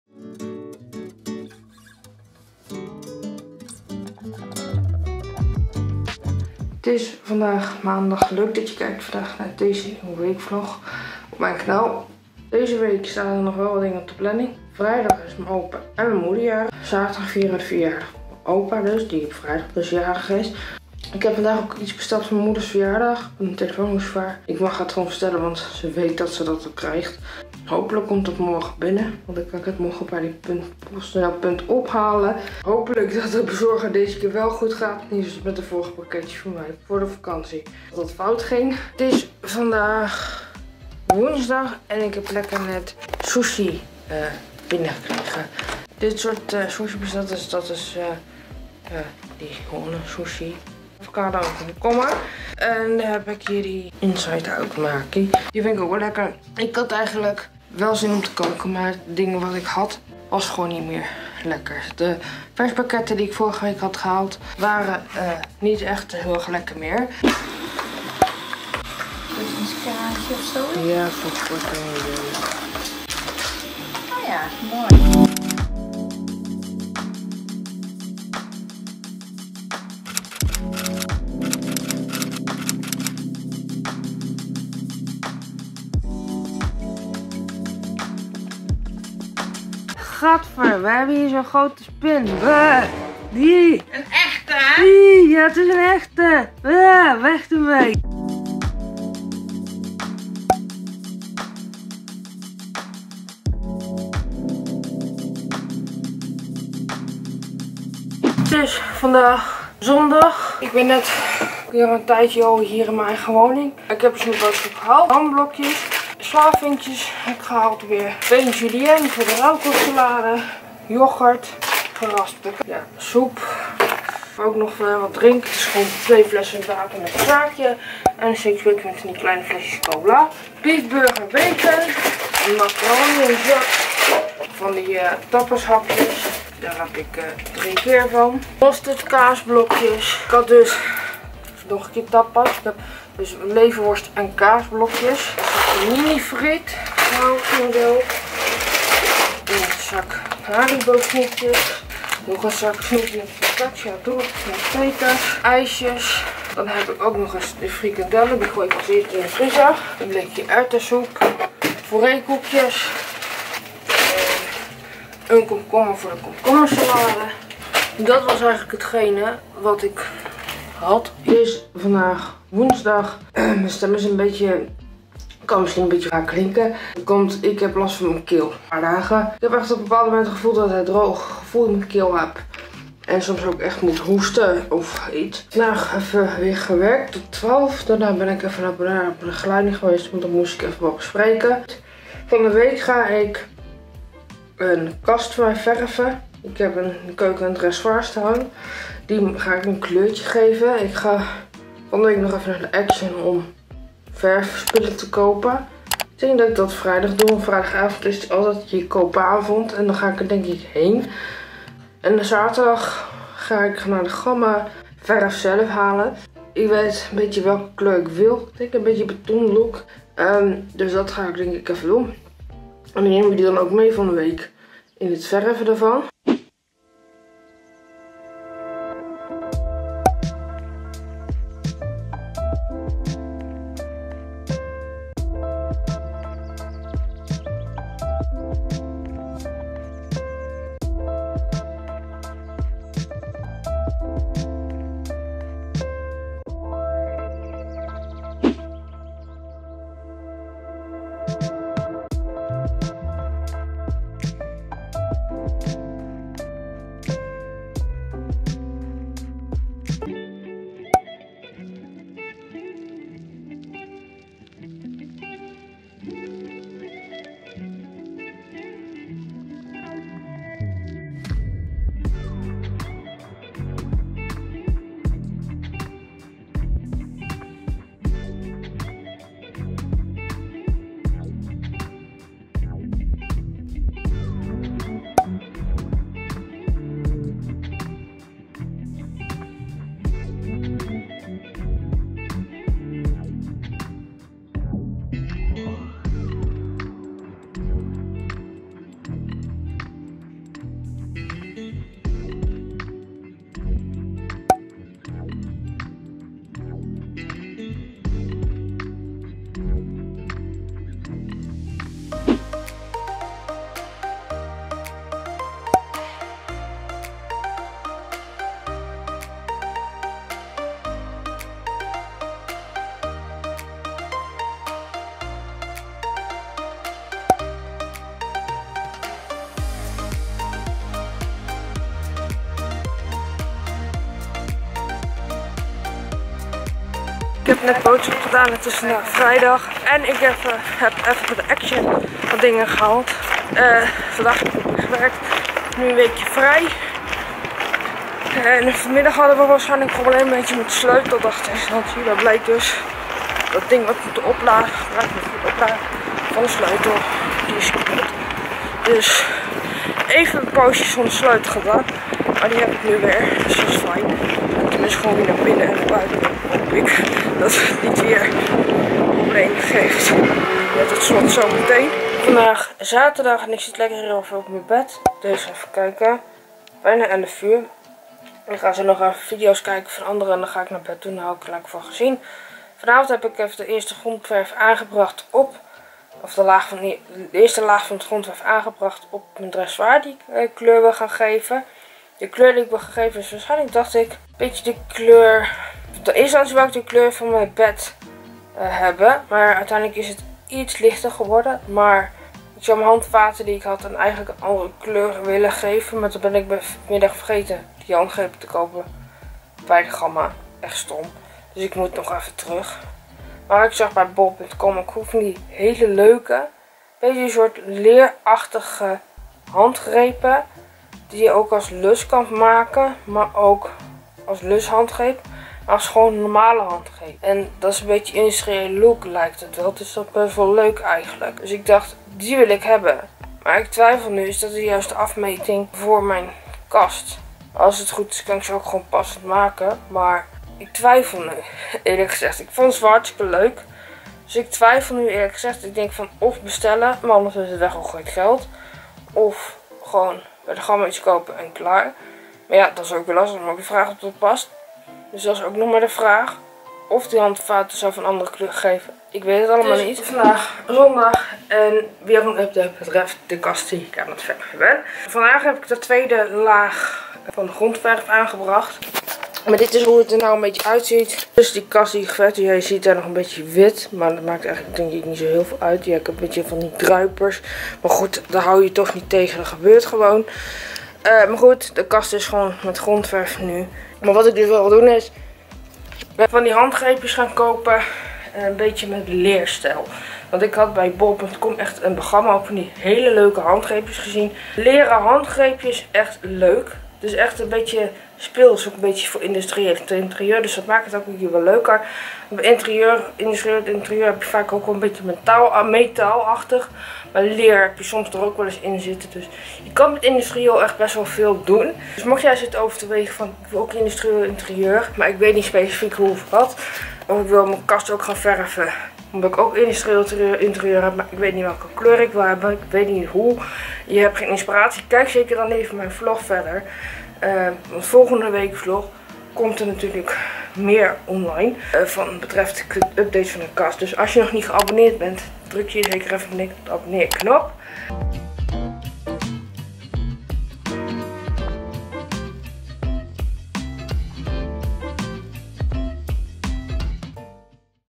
Weekvlog259:Sushi🍣, Verjaardagscadeau moeder🎁, Grote spin in huis😱,Shop with me Action🛍️
Hi, In deze weekvlog neem ik je mee in mijn dagelijkse leven met Autisme en een Licht verstandelijke beperking en mijn leven op de woongroep met Sushi, Verjaardagscadeau voor mijn moeder, Spin in huis en een shop with me.  Dank je wel voor het kijken! , tot de volgende vlog✨.

Klik op de notificatie bel om geen enkele vlog te missen. - Voor samenwerkingen neem contact op via: